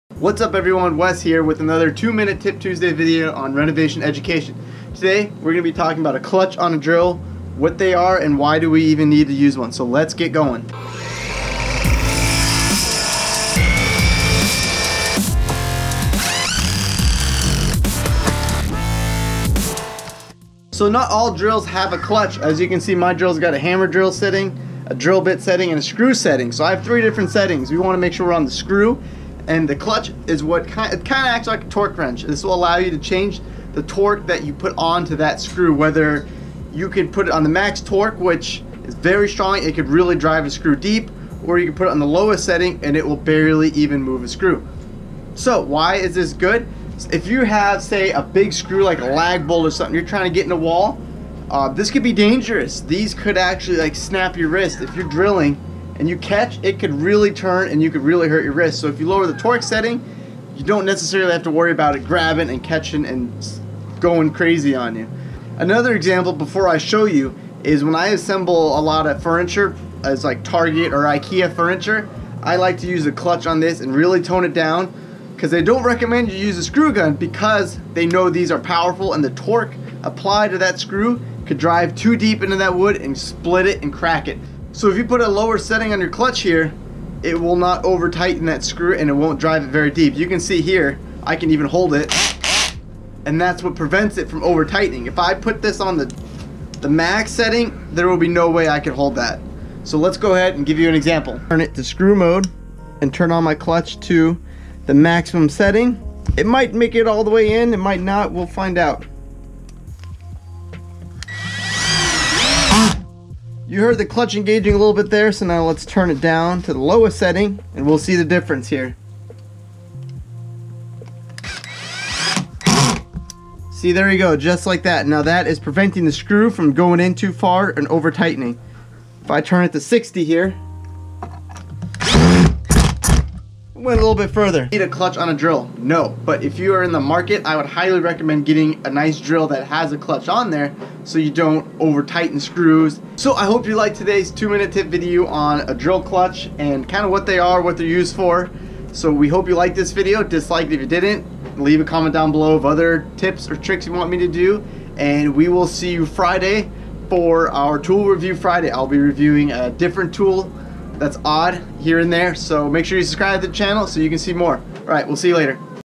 What is a Drill Clutch?
Welcome back to another two minute Tuesday video! In this video we explain what a drill clutch is and how to use it!

Ridgid cordless drill